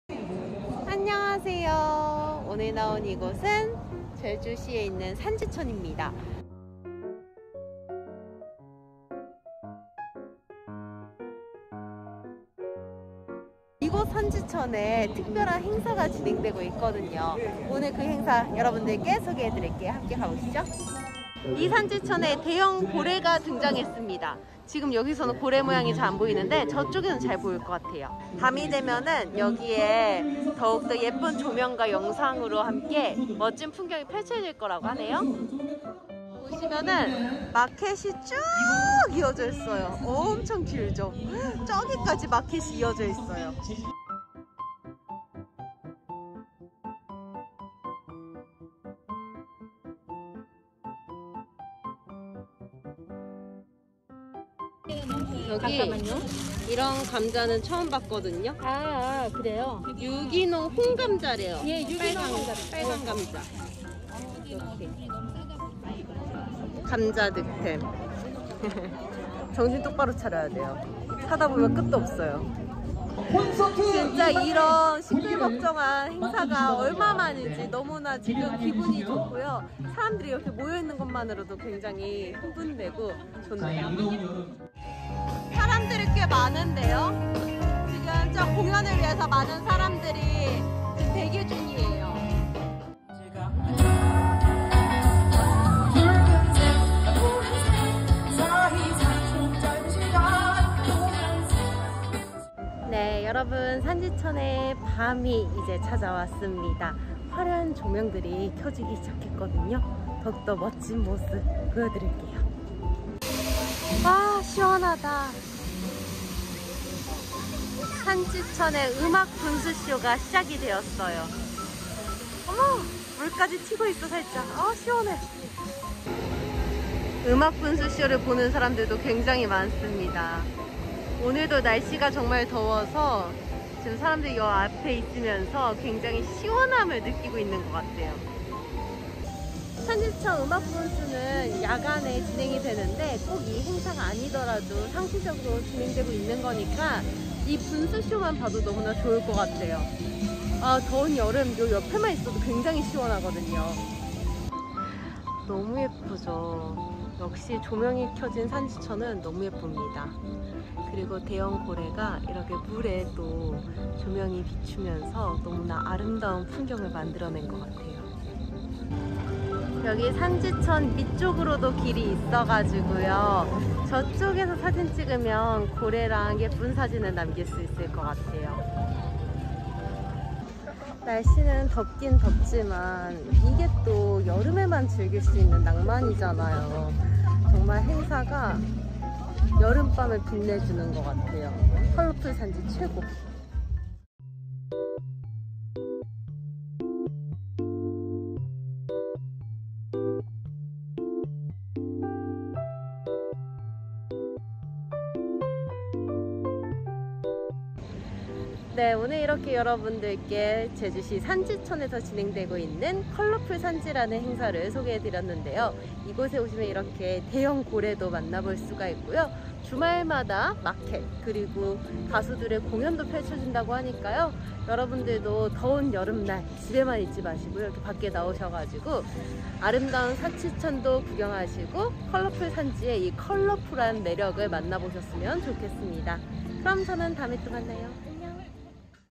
[📸] 산지천에서 열리는 한 여름의 축제｜컬러풀산지
☆ 셀프캠 장소 정보 ☆

장소 : 산지천
주소 : 제주 제주시 이도일동

[ LGU+ 185번 / olleh tv 162번 / SK브로드밴드 225번 / skylife 192번 ] 에서 만나 보세요